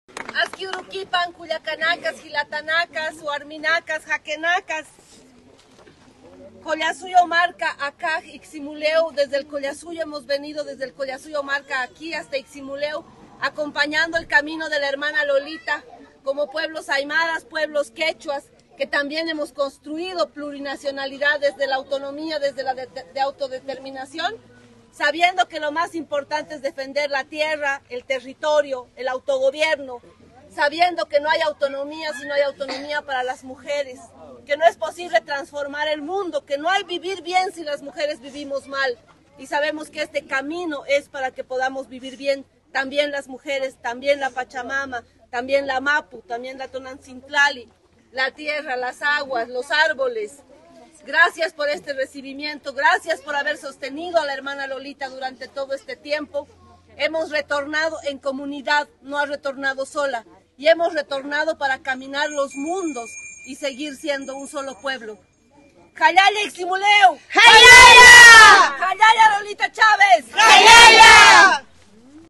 RetornoDelExilio 🌸✊🏽 Aura Lolita Chávez Ixcaquic regresó a su comunidad Santa Cruz del Quiché
RetornamosEnComunidad 🌸✊🏽 Aura Lolita Chávez Ixcaquic regresó a su comunidad Santa Cruz del Quiché

Después de siete años de exilio regresó acompañada de mujeres que también han luchado en sus comunidades. Con las palabras “Lolita no retorna sola”,  fue recibida esta mañana.

📹Julián Ventura